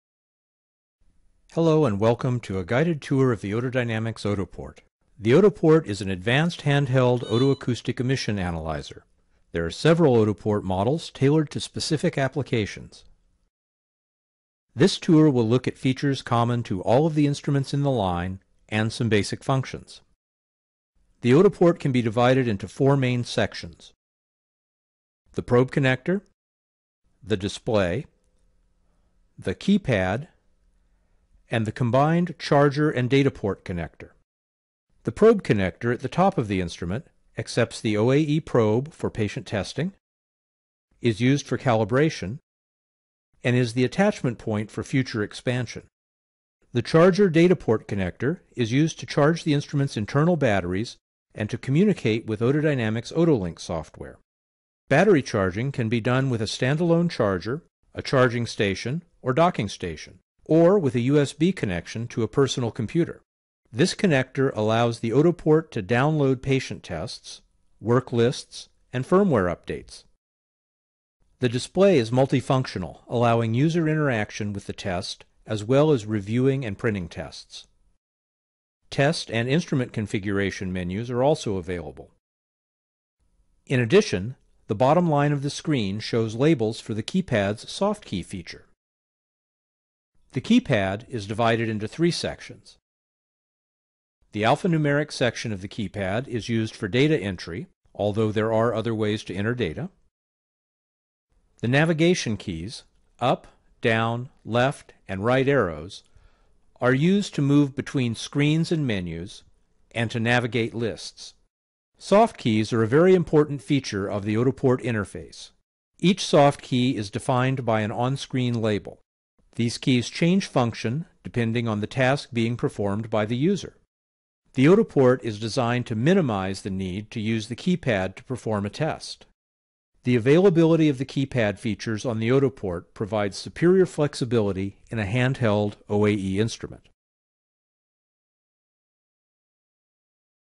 Otoport Tour
An introduction to Otodynamics' handheld Otoport OAE instrument